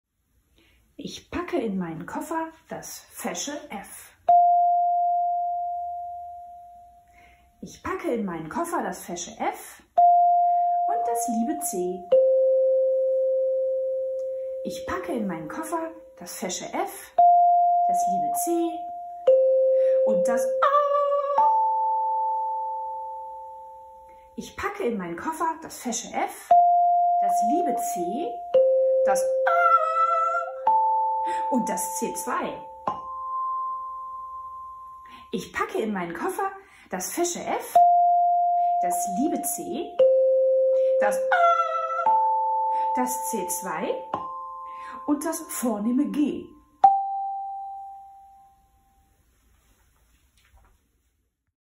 Spielend Noten lernen für Kinder | Floh im Ohr TV
Ich packe in meinen Koffer: die "Notenschnutzies" von Floh im Ohr TV! Du kennst die sprechenden Noten noch nicht? Dann schau unbedingt mal vorbei!
Spielend leicht Noten lernen!

Floh im Ohr TV mit Simone Ludwig